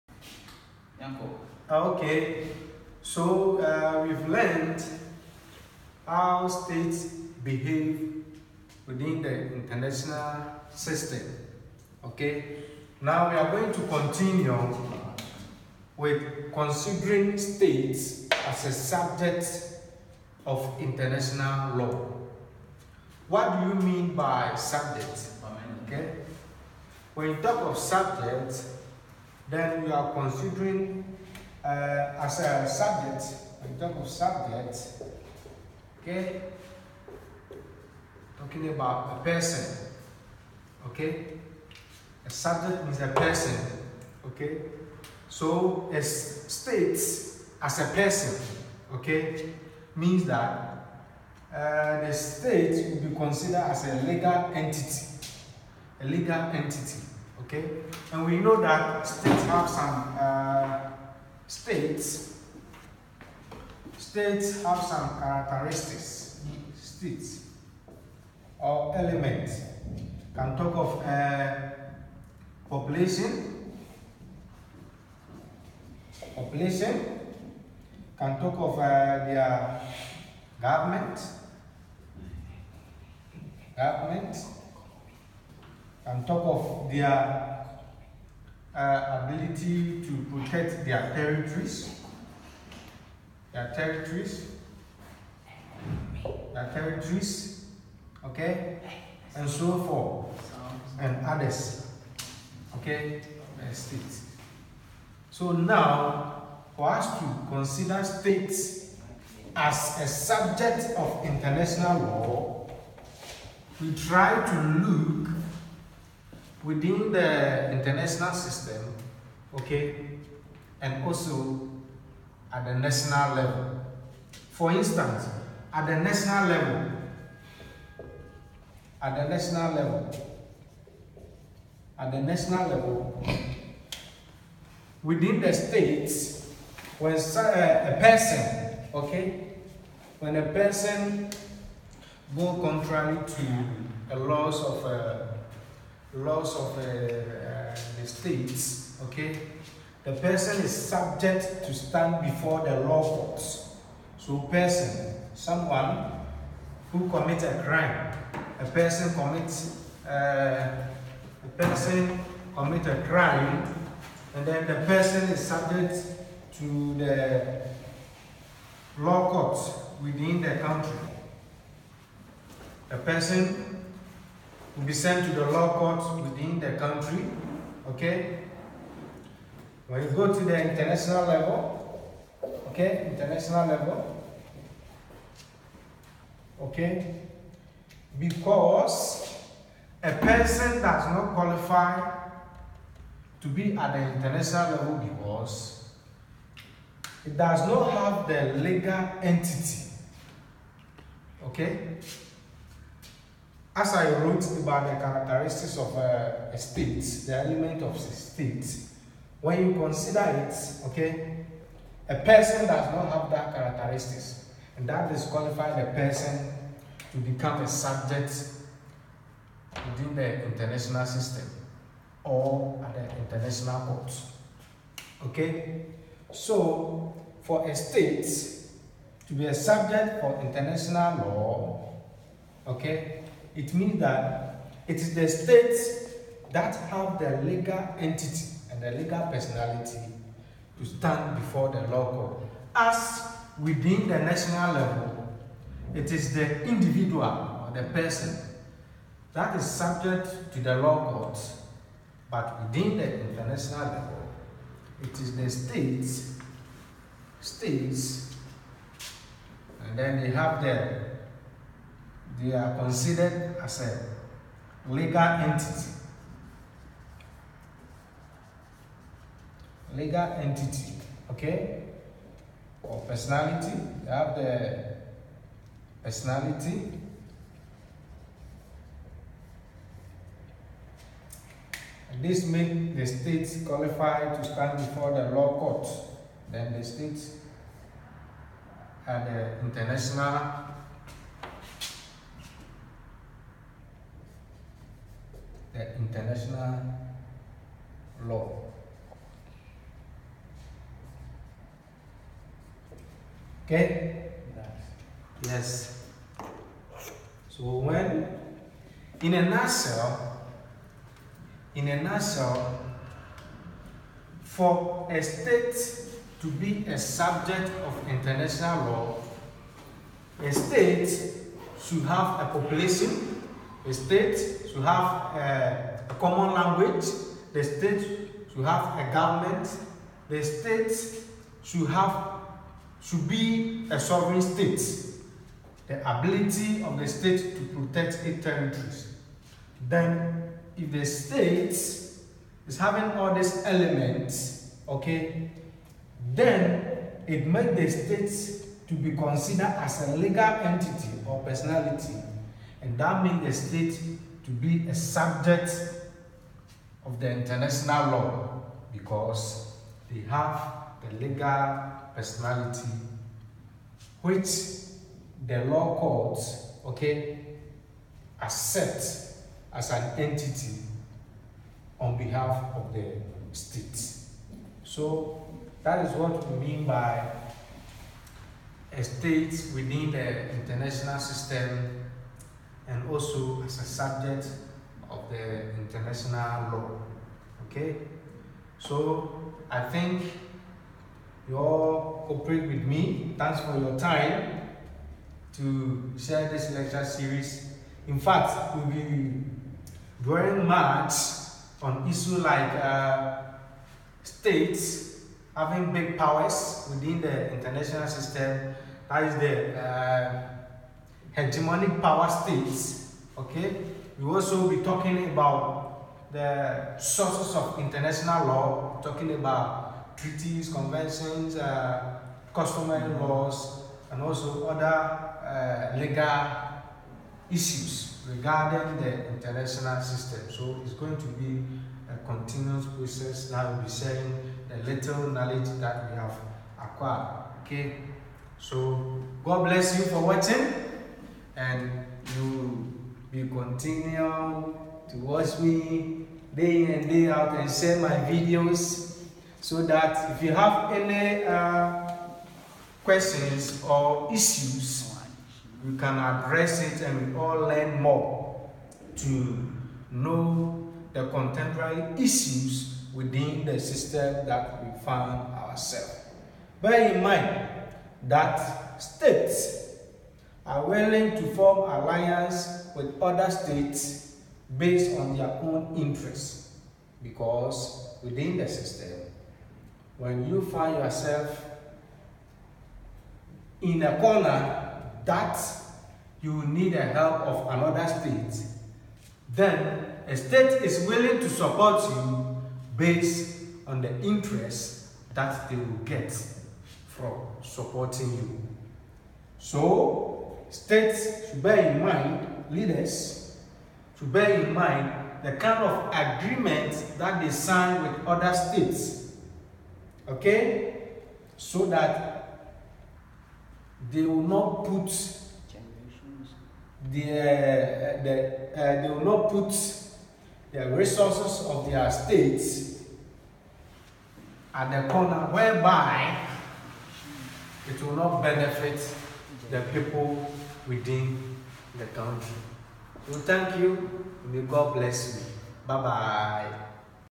Structural element of the international system (states)
Asamoah  Boateng Ghanaian MA student, talks on international system